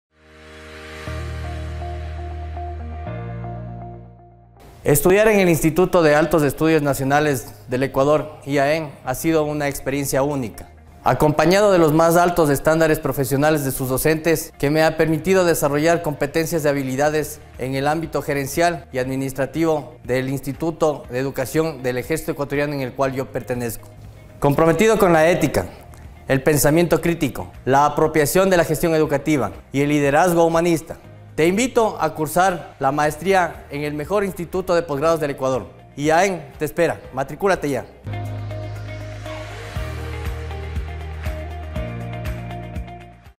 Maestría en Gestión Educativa - Oferta Académica Octubre 2022
La Escuela de Gobierno y Administración Pública, del Instituto de Altos Estudios Nacionales - IAEN, invita a postularse a la Maestría en Gestión Educativa.